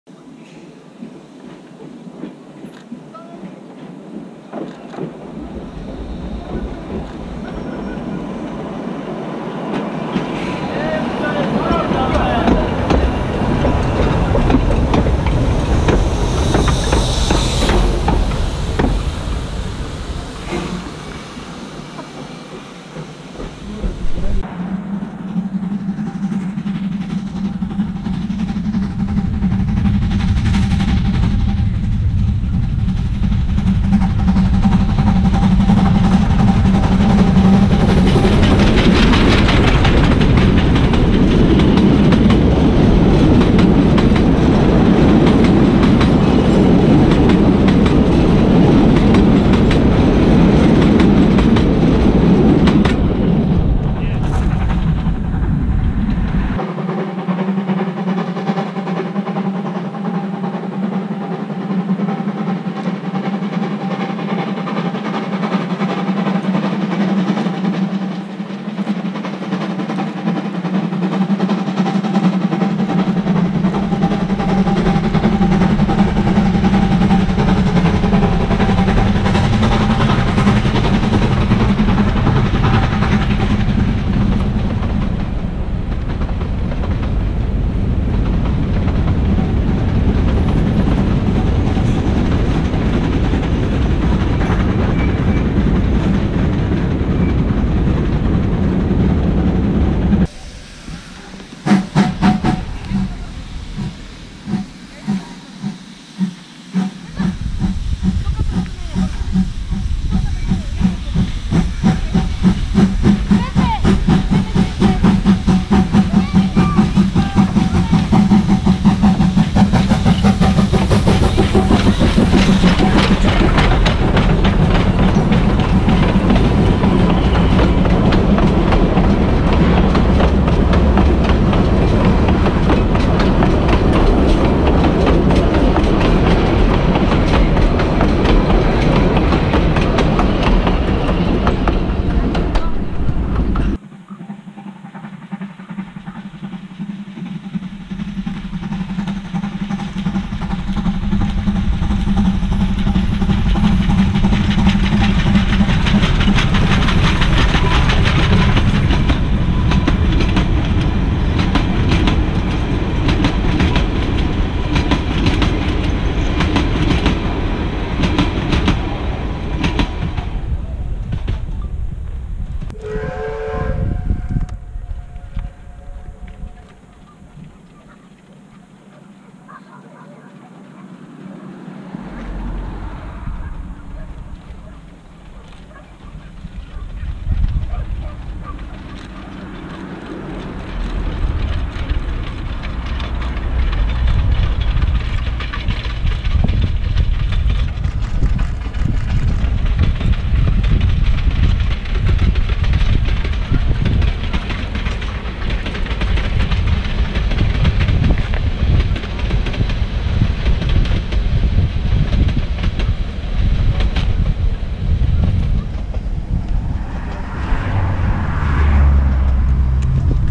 From the archives: Celebrating 70-th anniversary
Build in 1949 in Chrzanow locomotive factory in Poland, the narrow gauge steam locomotives 600 series celebrated their 70-th anniversary in Bulgaria in the autumn of 2019. Nowadays only one locomotive from the series is still in movement - 609.76. It was restored from the state passenger operator BDZ in 2004 and since then has been used for attraction trips on the 760mm narrow gauge line from Septemvri to Dobrinishte several times a year. The anniversary was celebrated with a trip with the steam locomotive from Septemvri to Velingrad on the 6-th of September, which is also the Unification Day of Bulgaria.